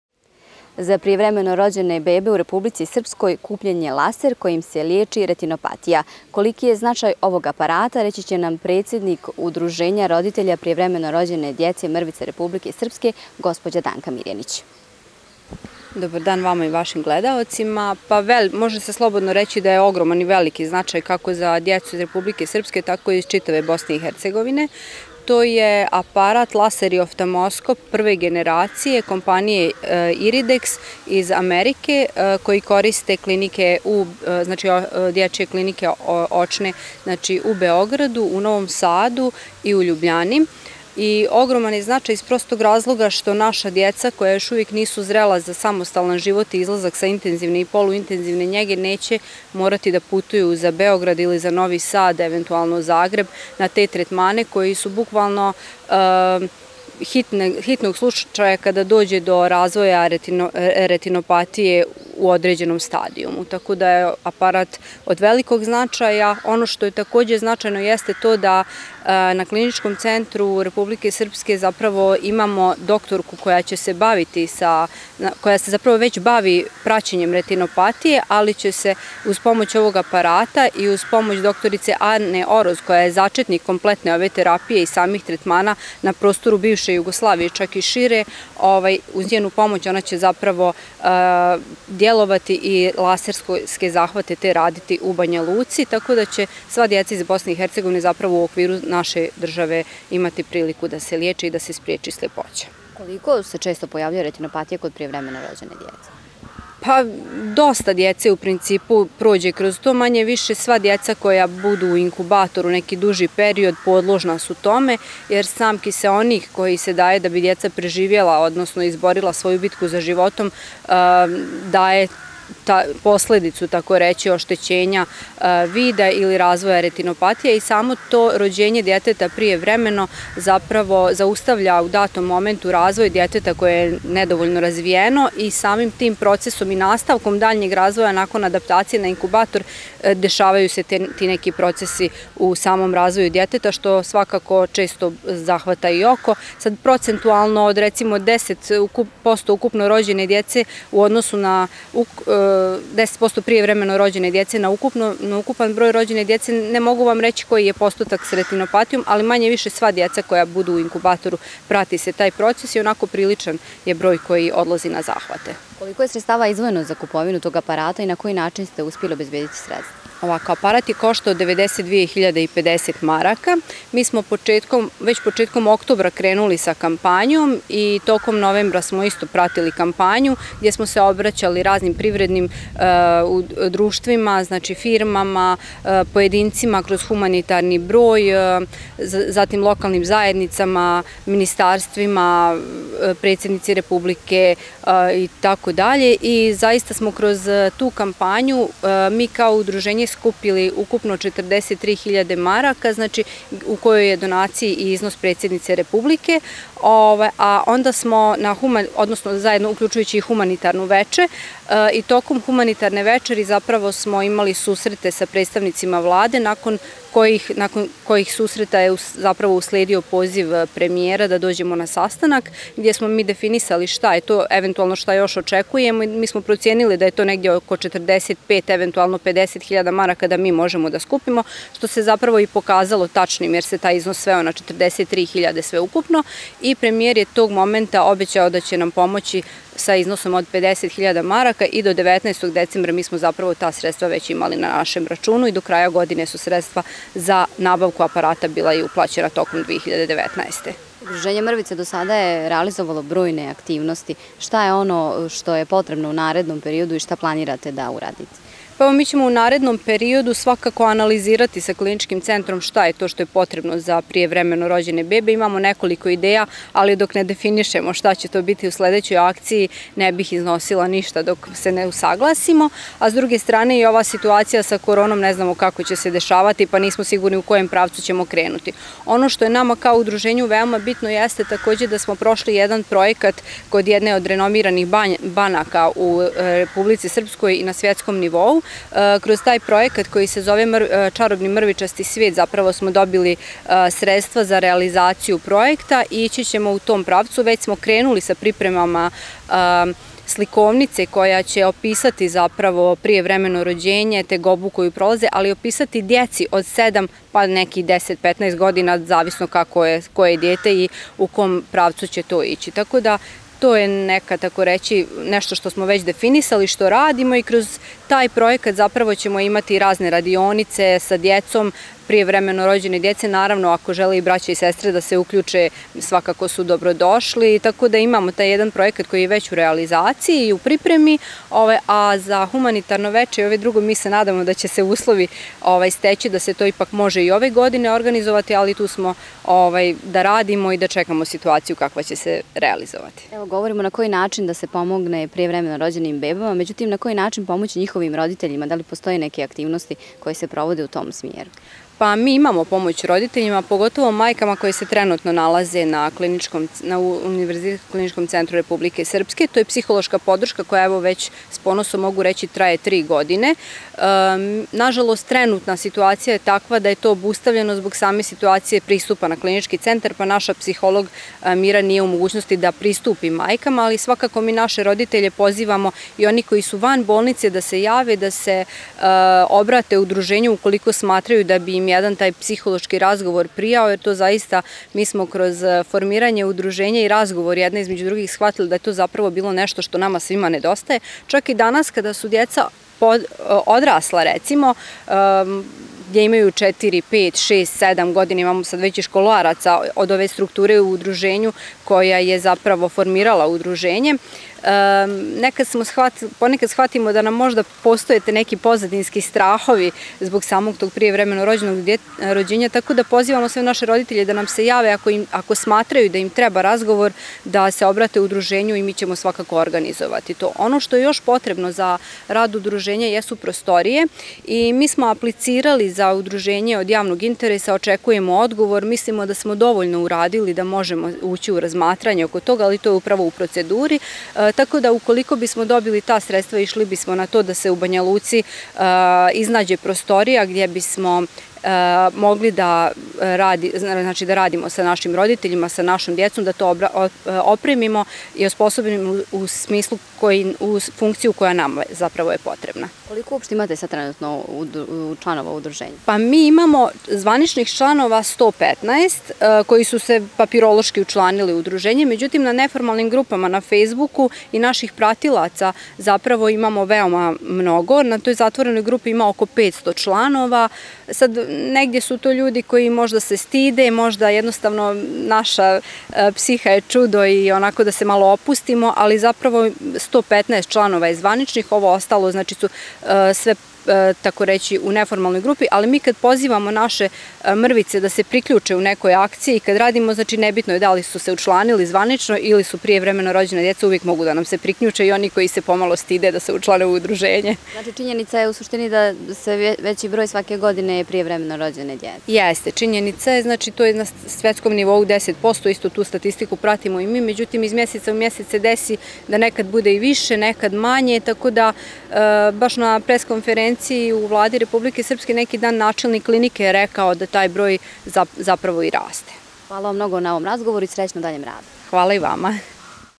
„Mrvicama“ aparat za liječenje retinopatije
Za prijevremeno rođene bebe u Republici Srpskoj kupljen je laser kojim se liječi retinopatija. Predsjednik Udruženja roditelja prijevremeno rođene djece „Mrvice“ Danka Mirjanić kaže da je aparat od velikog značaja kako za djecu Republike Srpske tako i za djecu iz čitave BiH.